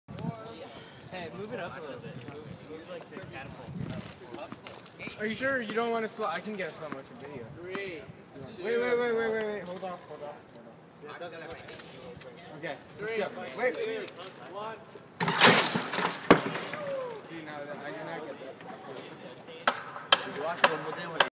20140327 Spring Fling Mean Green Machine Rapid Refiring
John Meehan, Isaac Smith, and Mark Fritz catapult consistently hit the 30 meter mark and could be reloaded and refired within 10 seconds.
Spring Fling at Suncoast. For the physics project, students have chosen their teams and are building their catapults outside of class from materials they find around the house or in their yards. Teams have a $50 spending cap, and Landstrom encourages them to use everyday materials, such as bungee cords, wood scraps, tires and rope. Students have prepared in class by studying catapult design and by discussing the mathematics and physics of getting an object to move through space. The teams are required to submit a catapult blueprint along with a list of the materials they will use.